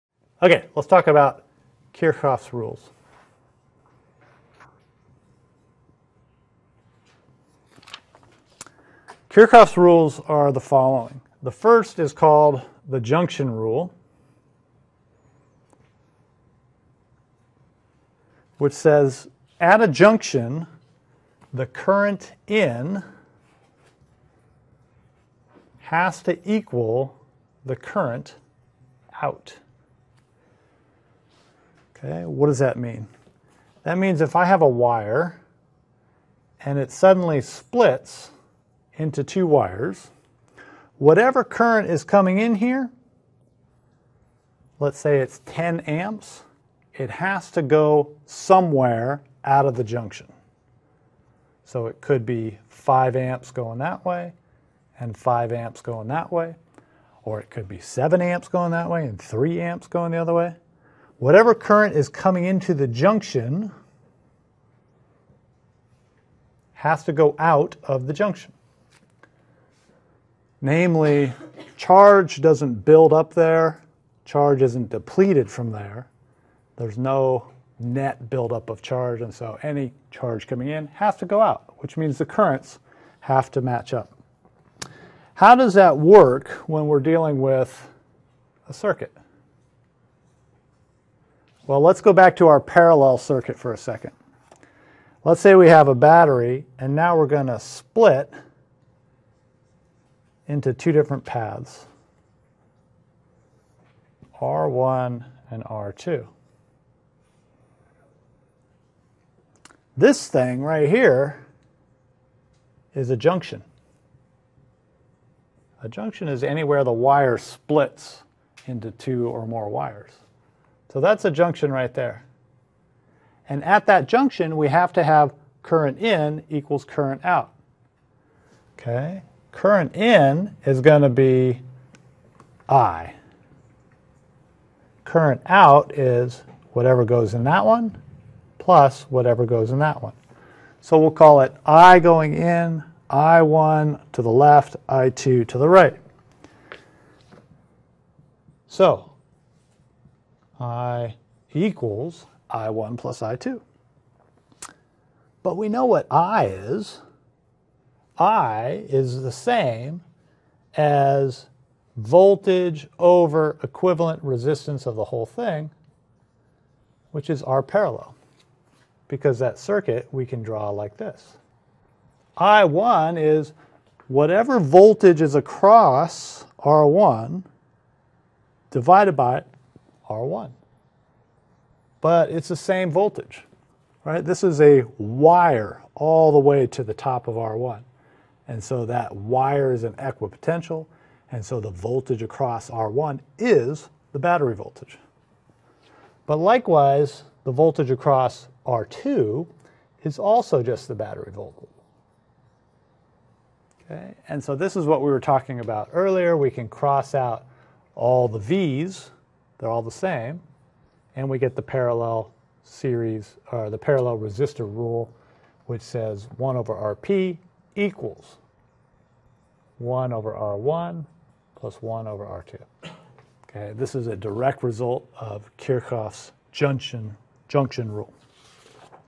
Kirchoff's Junction Rule | Physics with Professor Matt Anderson | M22-06
Current in = current out.  This is really a statement about conservation of charge.  If it doesn't build up anywhere, then at a junction (multiple wires joining together) the charge has to go somewhere.